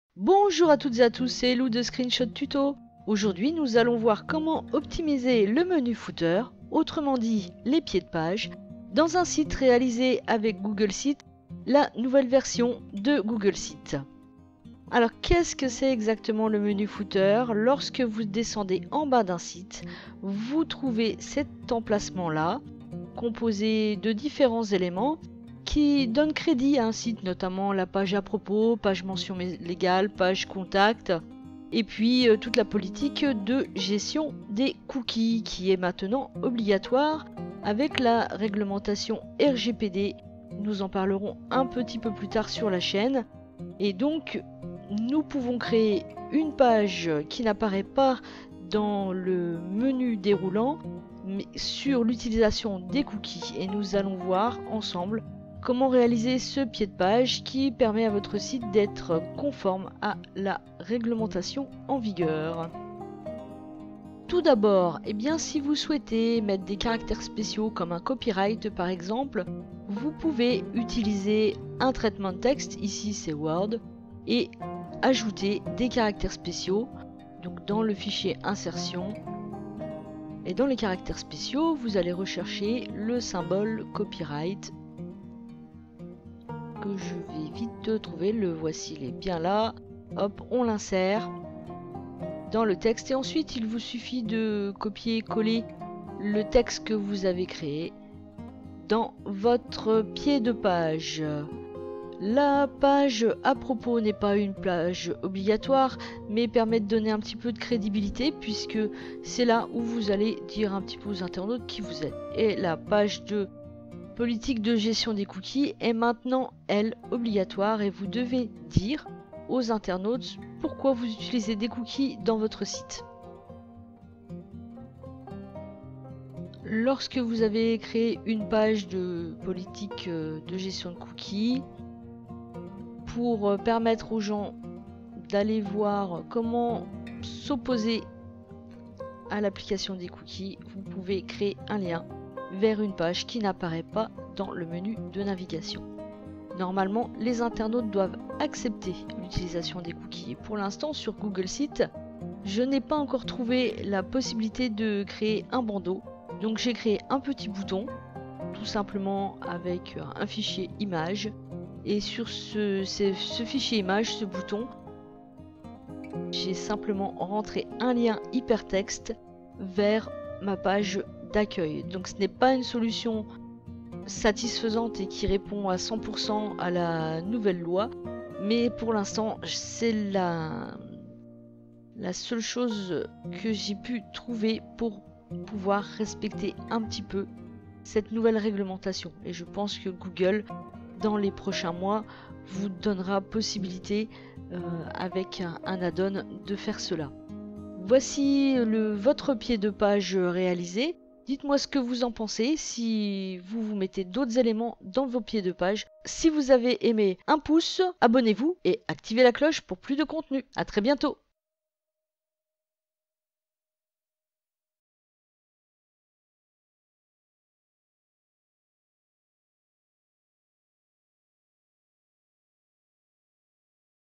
🌐 [Tuto] Comment optimiser le menu footer dans
Le menu footer ou pied de page, donne de la crédibilité à votre site. Vous pouvez y mettre des liens vers les pages obligatoire comme les mentions légales ou la politique d'utilisation des cookies ou des pages optionnelle mais utiles comme la page à propos, ou la page contact.

🎼 CREDITS MUSICAUX
Artiste : __url_artiste__